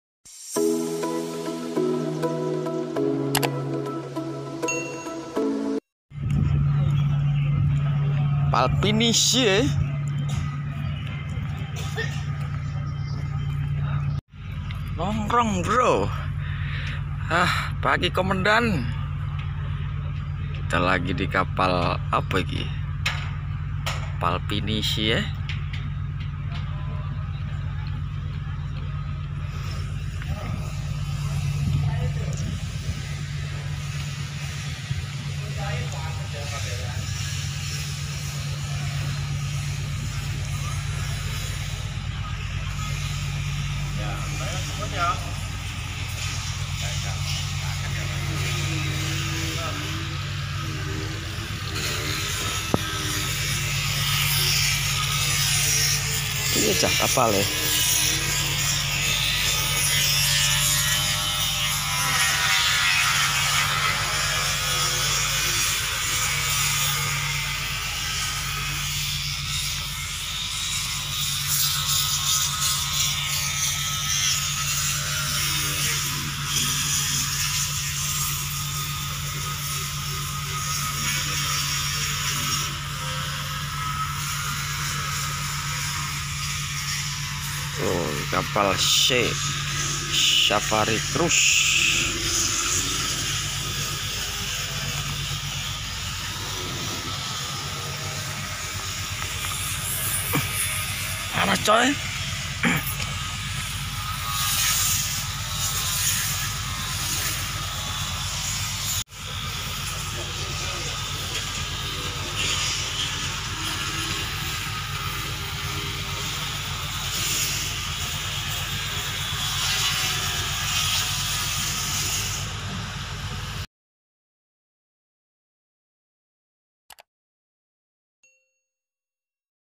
KAPAL PINISINYA INDONESIA ( Sea Safari 9 ) BERSANDAR DI PELABUHAN BENOA BALI
kapal pinisinya Indonesia lbersandar di pelan Han Benoa bali